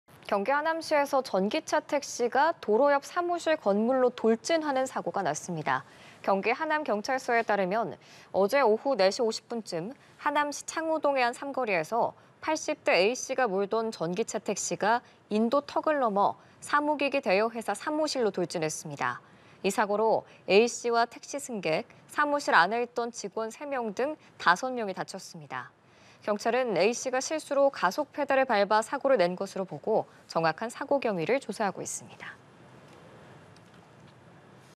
경기 하남서 전기차 택시 건물 돌진…5명 부상 / 연합뉴스TV (YonhapnewsTV)
경기 하남서 전기차 택시 건물 돌진…5명 부상

경기 하남시에서 전기차 택시가 도로 옆 사무실 건물로 돌진하는 사고가 났습니다.

경기 하남경찰서에 따르면 어제(22일) 오후 4시 50분쯤 하남시 창우동의 한 삼거리에서 80대 A씨가 몰던 전기차 택시가 인도 턱을 넘어 사무기기 대여 회사 사무실로 돌진했습니다.

이 사고로 A씨와 택시 승객, 사무실 안에 있던 직원 3명 등 5명이 다쳤습니다.

경찰은 A씨가 실수로 가속페달을 밟아 사고를 낸 것으로 보고 정확한 사고 경위를 조사하고 있습니다.